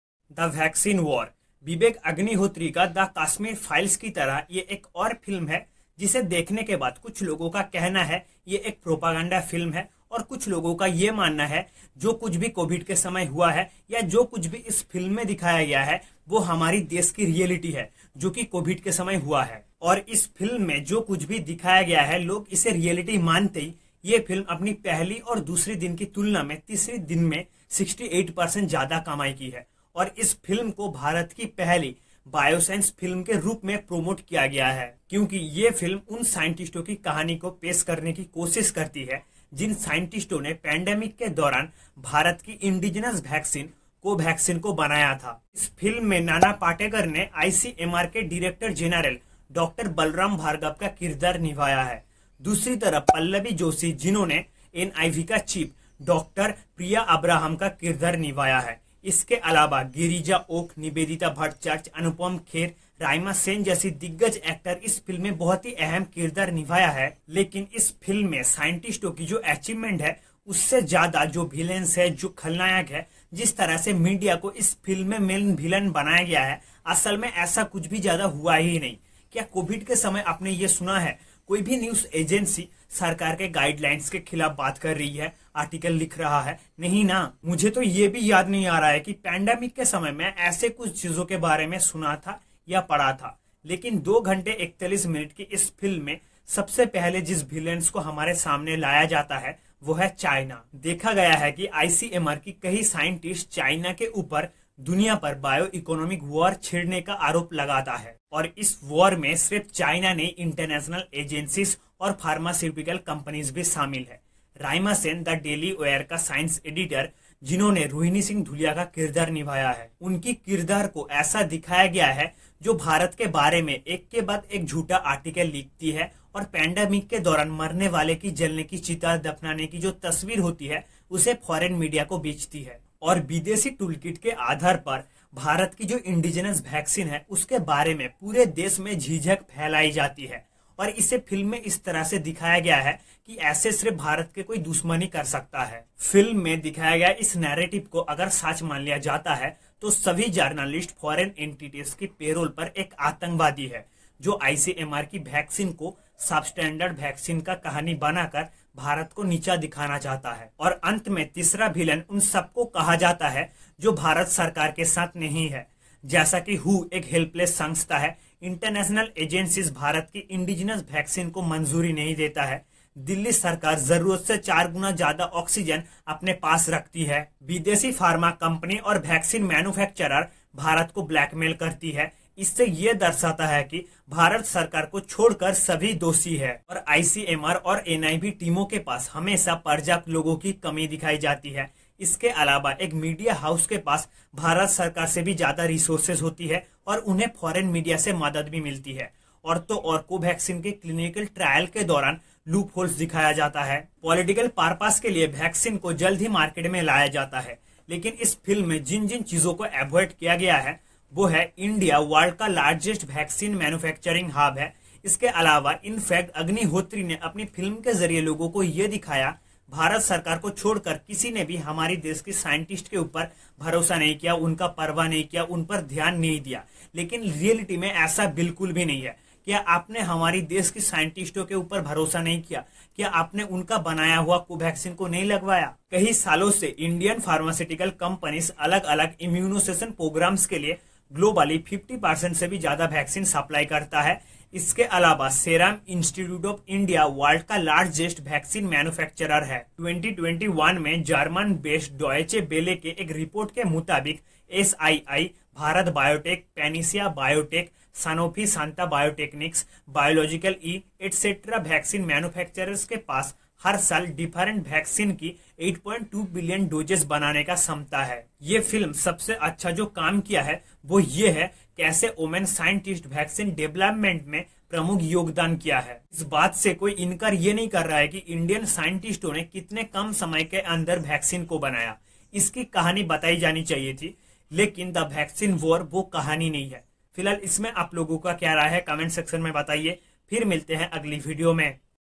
'The Vaccine War' Honest Review : This is not the story of our scientists
Vivek Agnihotri's The Vaccine War is finally in the theatres - after the massive success of The Kashmir Files, Agnihotri again picks up an emotive issue. But is the very basis of the movie based on facts - or a brazen lie?

In this detailed Deshbhakt review - we talk about the four legs this movie stands on. and why its important to remember that facts are facts.